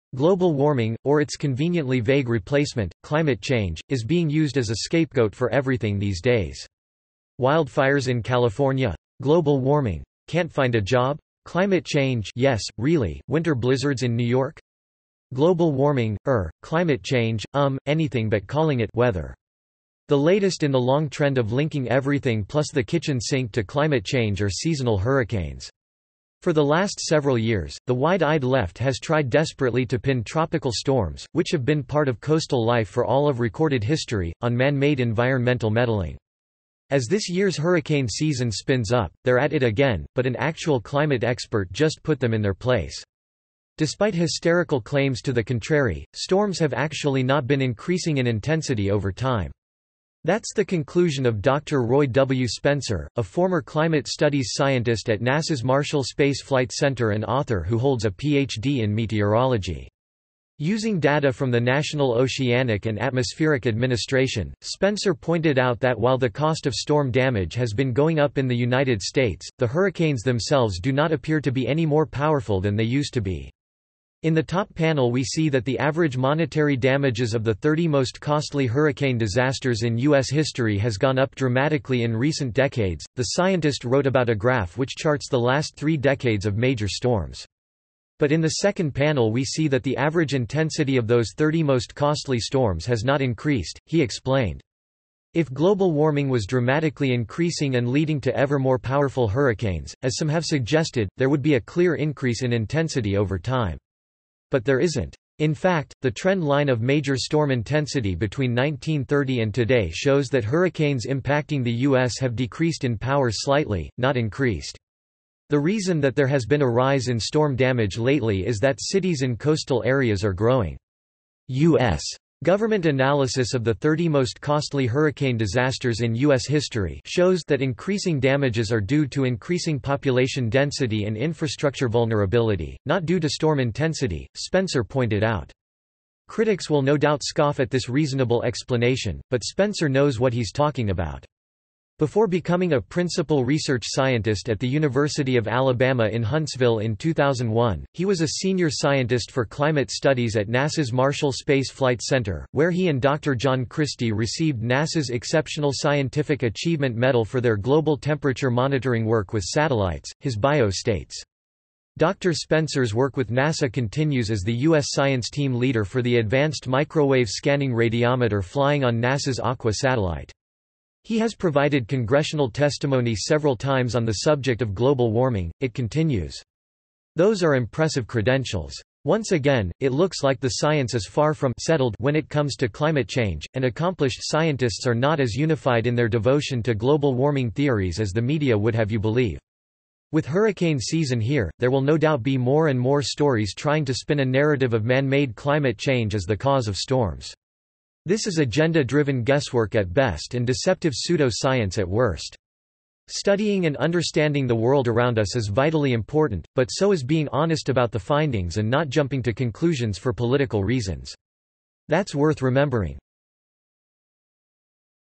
Expert Blows Apart Biggest Argument Libs Use To Link Hurricanes & Global Warming by Finest news
Welcome to Finest News.
Global warming — or its conveniently vague replacement, “climate change” — is being used as a scapegoat for everything these days.

Wildfires in California? Global warming.

Can’t find a job? Climate change (yes, really.)

Winter blizzards in New York? Global warming, er, climate change, um, anything but calling it “weather.”

The latest in the long trend of linking everything plus the kitchen sink to climate change are seasonal hurricanes.

For the last several years, the wide-eyed left has tried desperately to pin tropical storms — which have been part of coastal life for all of recorded history — on man-made environmental meddling. As this year’s hurricane season spins up, they’re at it again, but an actual climate expert just put them in their place.

Despite hysterical claims to the contrary, storms have actually not been increasing in intensity over time.

That’s the conclusion of Dr. Roy W. Spencer, a former climate studies scientist at NASA’s Marshall Space Flight Center and author who holds a Ph.D. in meteorology.

Using data from the National Oceanic and Atmospheric Administration, Spencer pointed out that while the cost of storm damage has been going up in the United States, the hurricanes themselves do not appear to be any more powerful than they used to be.

“In the top panel we see that the average monetary damages of the 30 most costly hurricane disasters in U.S history has gone up dramatically in recent decades,” the scientist wrote about a graph which charts the last three decades of major storms.

“But in the 2nd panel we see that the average intensity of those 30 most costly storms has not increased,” he explained.

If global warming was dramatically increasing and leading to ever more powerful hurricanes, as some have suggested, there would be a clear increase in intensity over time. But there isn’t.

In fact, the trend line of major storm intensity between 1930 and today shows that hurricanes impacting the U.S. have decreased in power slightly, not increased.

The reason that there has been a rise in storm damage lately is that cities in coastal areas are growing.

“U.S. Government analysis of the 30 most costly hurricane disasters in U.S. history (shows) that increasing damages are due to increasing population density and infrastructure vulnerability, not due to storm intensity,” Spencer pointed out.

Critics will no doubt scoff at this reasonable explanation, but Spencer knows what he’s talking about.

“Before becoming a Principal Research Scientist at the University of Alabama in Huntsville in 2001, he was a Senior Scientist for Climate Studies at NASA’s Marshall Space Flight Center, where he and Dr. John Christy received NASA’s Exceptional Scientific Achievement Medal for their global temperature monitoring work with satellites,” his bio states.

“Dr. Spencer’s work with NASA continues as the U.S. Science Team leader for the Advanced Microwave Scanning Radiometer flying on NASA’s Aqua satellite. He has provided congressional testimony several times on the subject of global warming,” it continues.

Those are impressive credentials. Once again, it looks like the science is far from “settled” when it comes to climate change, and accomplished scientists are not as unified in their devotion to global warming theories as the media would have you believe.

With hurricane season here, there will no doubt be more and more stories trying to spin a narrative of man-made climate change as the cause of storms.

This is agenda-driven guesswork at best and deceptive pseudo-science at worst. Studying and understanding the world around us is vitally important… but so is being honest about the findings and not jumping to conclusions for political reasons. That’s worth remembering.